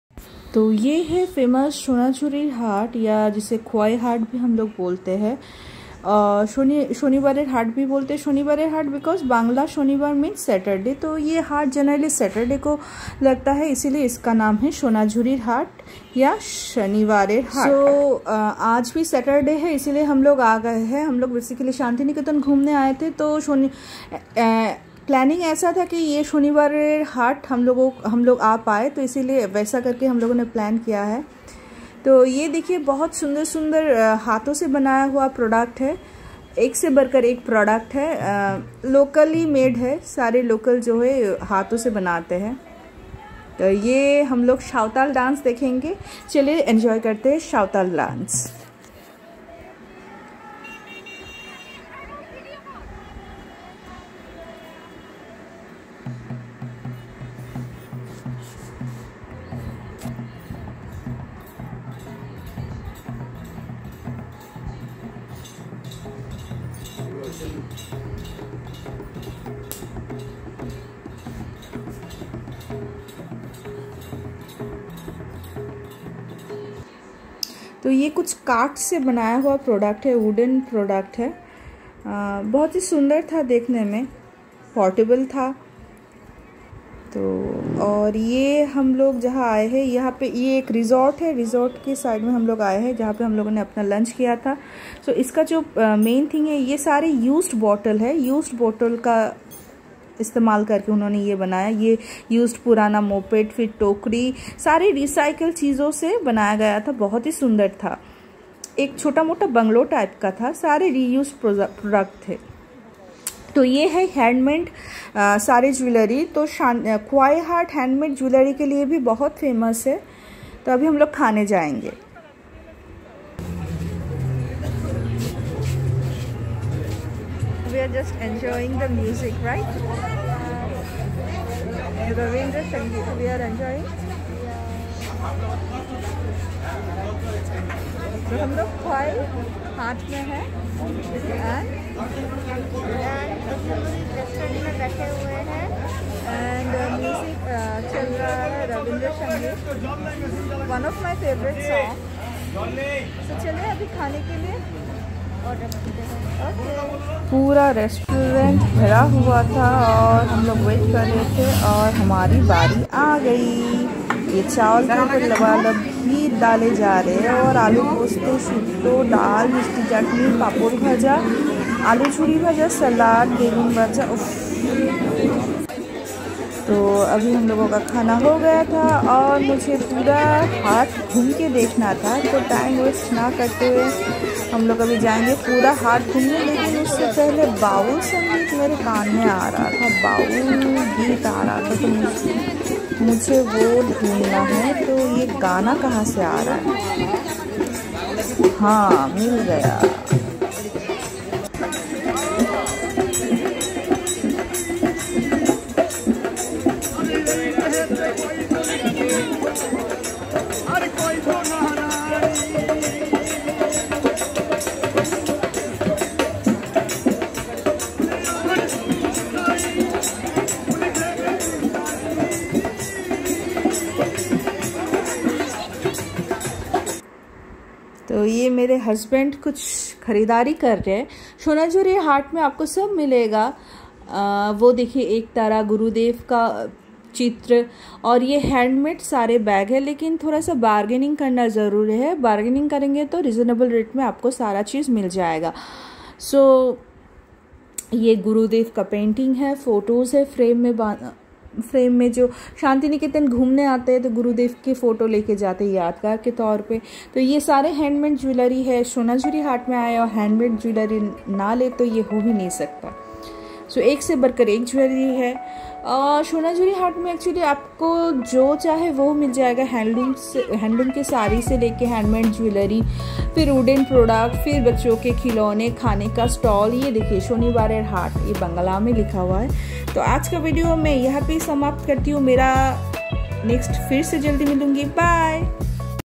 Shantiniketan Vlog Part 3
Exploring and sending time in the famous Saturday haat or bazaar of Shantiniketan